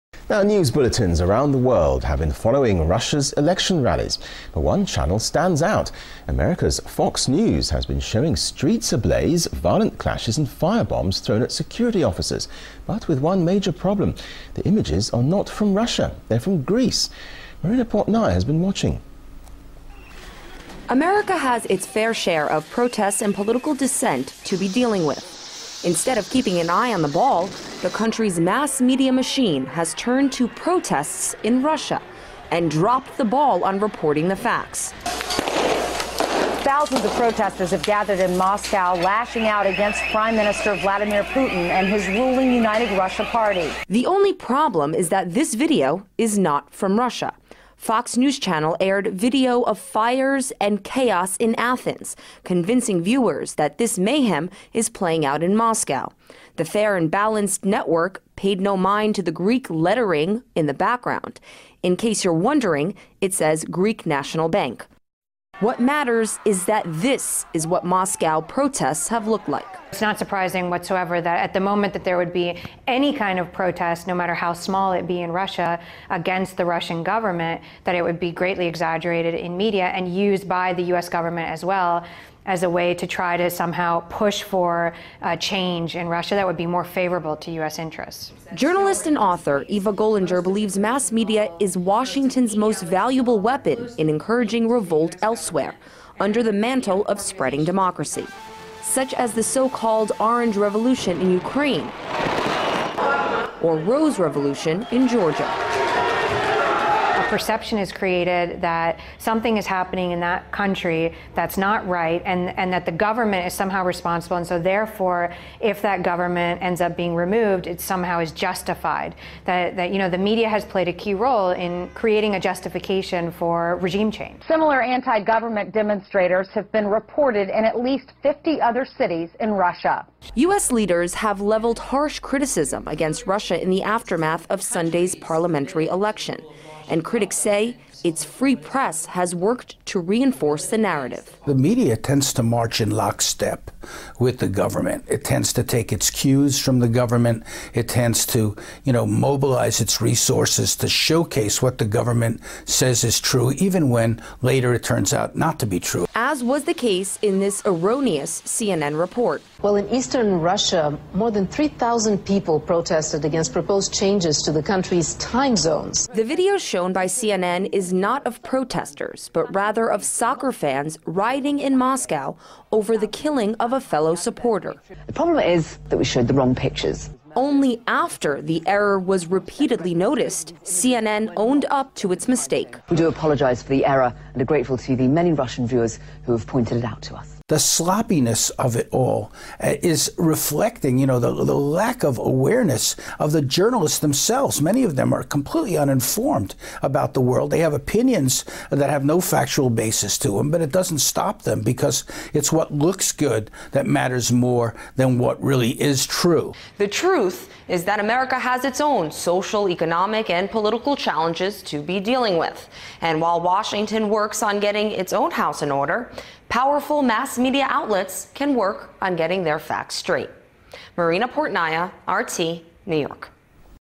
FOX News Caught Using Fake Video Of Riots
News bulletins around the world have been following Russia's election rallies. But one channel stands out - America's Fox News has been showing streets ablaze, violent clashes and firebombs thrown at security officers, but with one major problem - the images are not from Russia, they're from Greece!